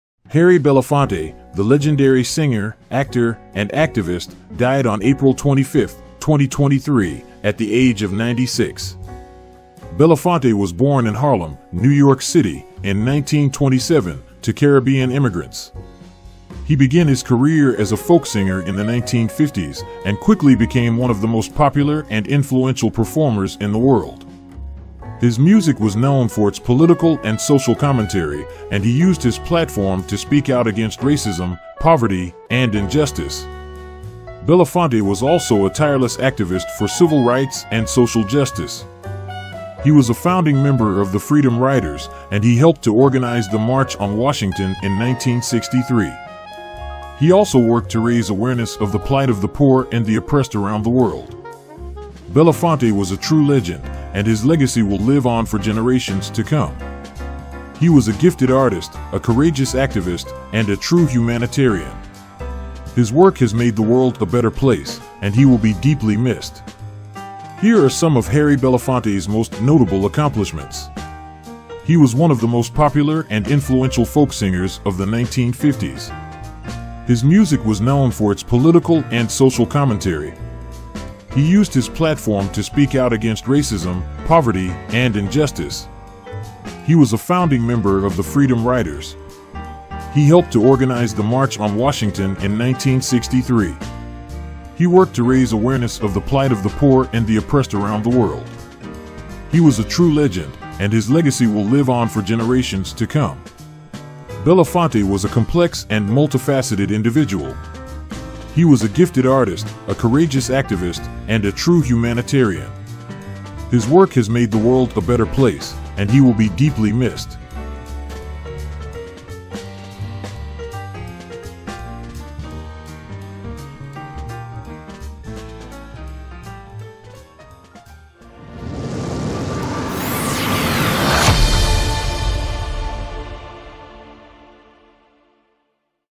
R.I.P. Harry Belafonte: March 1, 1927 - April 25, 2023 - AI Generated Images of Painted Harlem
Article by Google Bard
AI Generated Images by Adobe Firefly
Tyler AI Generated Voice Over by Revoicer

Harry Belafonte, the legendary singer, actor, and activist, died on April 25, 2023, at the age of 96. Belafonte was born in Harlem, New York City, in 1927, to Caribbean immigrants. He began his career as a folk singer in the 1950s, and quickly became one of the most popular and influential performers in the world. His music was known for its political and social commentary, and he used his platform to speak out against racism, poverty, and injustice.

Belafonte was also a tireless activist for civil rights and social justice. He was a founding member of the Freedom Riders, and he helped to organize the March on Washington in 1963. He also worked to raise awareness of the plight of the poor and the oppressed around the world.

Belafonte was a true legend, and his legacy will live on for generations to come. He was a gifted artist, a courageous activist, and a true humanitarian. His work has made the world a better place, and he will be deeply missed.

Here are some of Harry Belafonte's most notable accomplishments:

He was one of the most popular and influential folk singers of the 1950s.
His music was known for its political and social commentary.
He used his platform to speak out against racism, poverty, and injustice.
He was a founding member of the Freedom Riders.
He helped to organize the March on Washington in 1963.
He worked to raise awareness of the plight of the poor and the oppressed around the world.
He was a true legend, and his legacy will live on for generations to come.
Belafonte was a complex and multifaceted individual. He was a gifted artist, a courageous activist, and a true humanitarian. His work has made the world a better place, and he will be deeply missed.